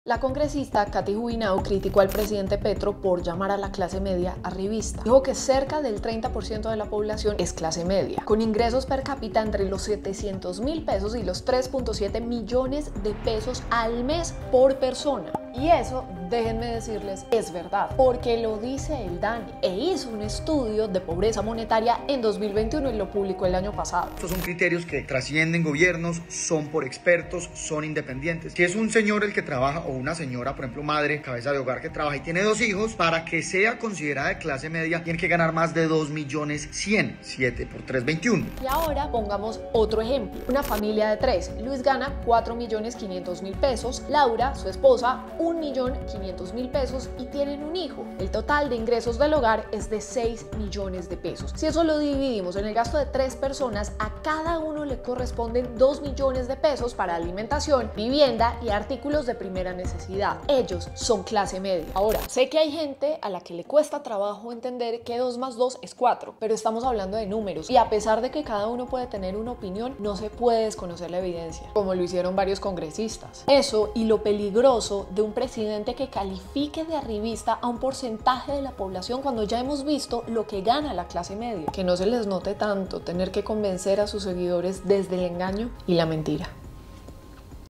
¿Clase media arribista? Juanita Gómez opina sobre la declaración de Petro.
El presidente Gustavo Petro desató una polémica al señalar a la clase media colombiana de "arribista", pero a esa clase media le cuesta llegar a fin de mes. Esta es la opinión de Juanita Gómez.